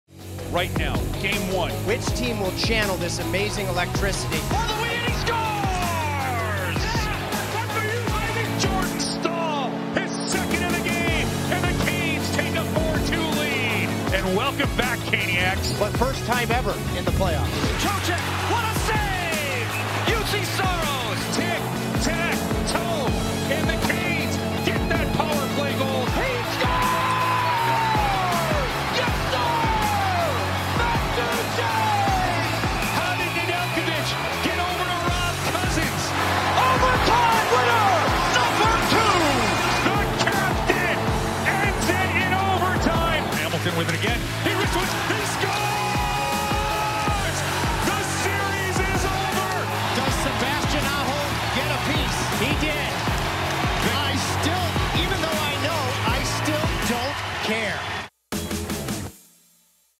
PPG Colorful Moments: Carolina Hurricanes vs. Nashville Predators
Check out some of the most colorful moments from the 1st Round matchup between the Carolina Hurricanes and Nashville Predators, presented by PPG.

Feeling social?